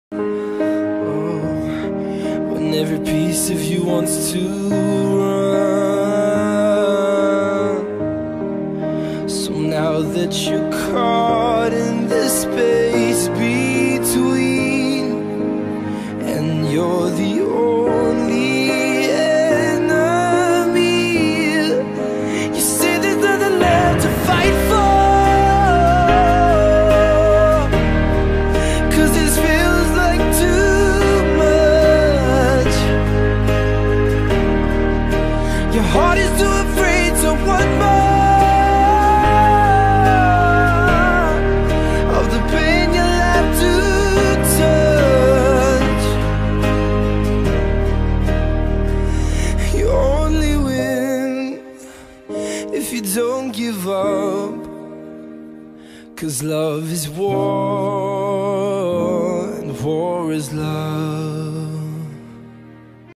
GREY STREET [h.s] | akdc
Trailer for the wonderful story, Grey Street, written by sawsee4 on wattpad!

 - andrea x